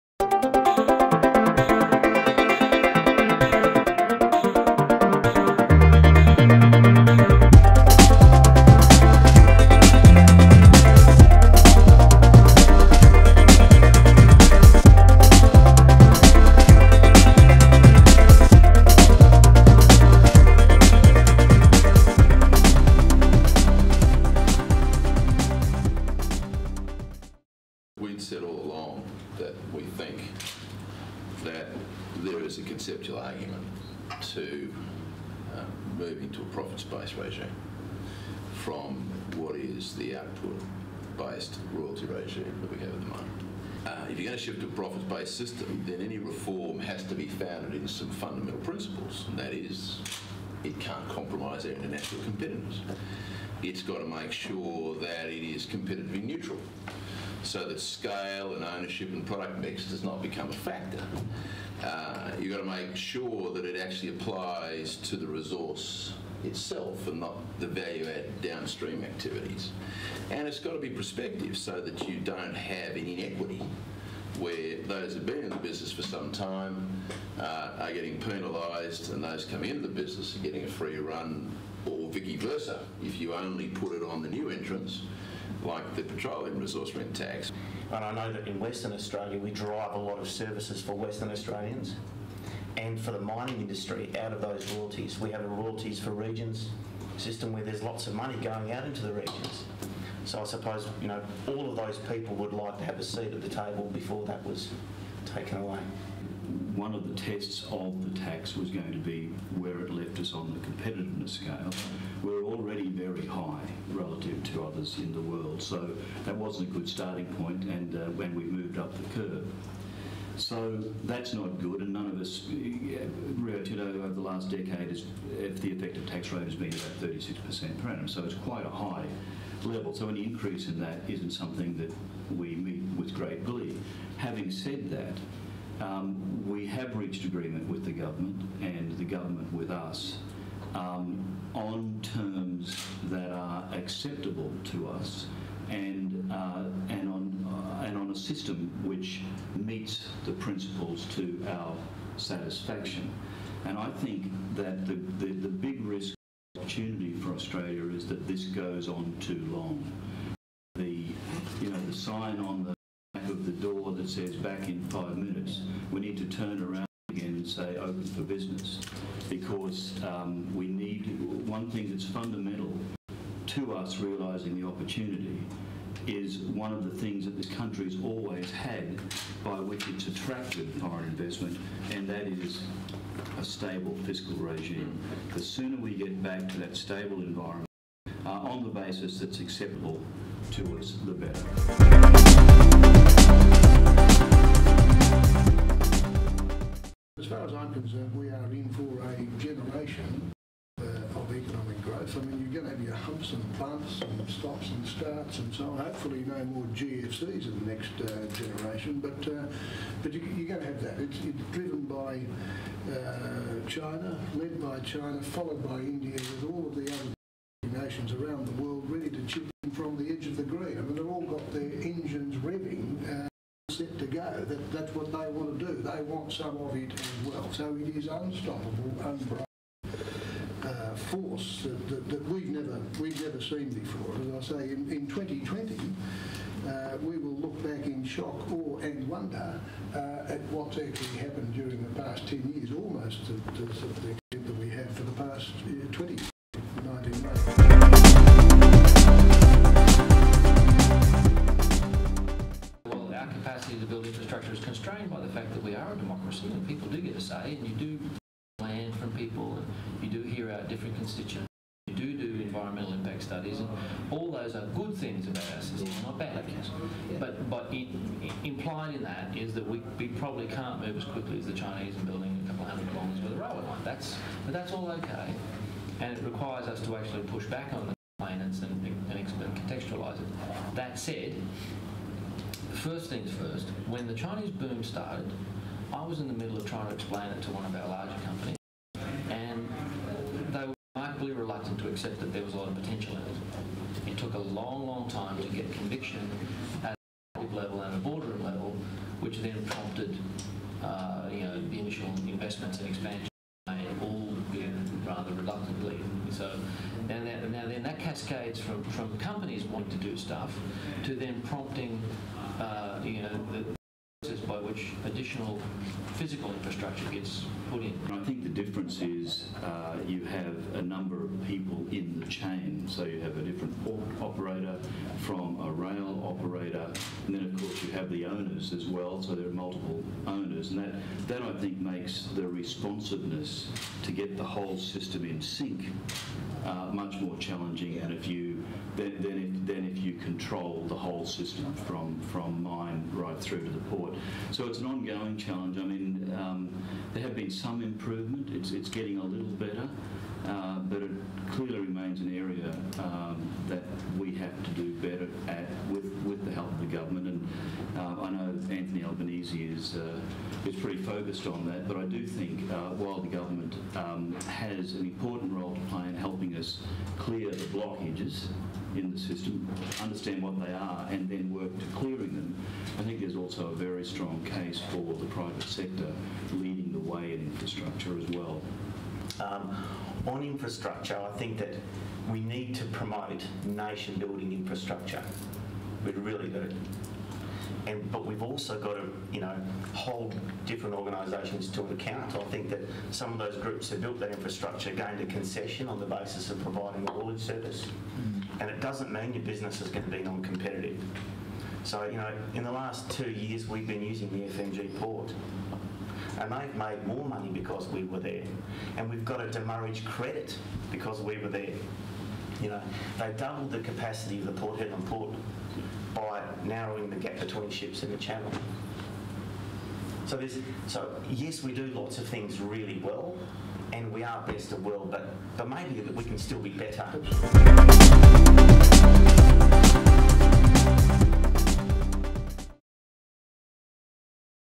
Smart Business Series resource debate
The 2010 Smart Business Series looks at the future of the health care, banking and resources industries takes a fresh look at infrastructure on Business Insight,

The fourth forum has five of Australia's top mining executives discussing how to make the most of the resources boom.

This content was sourced from the Australian.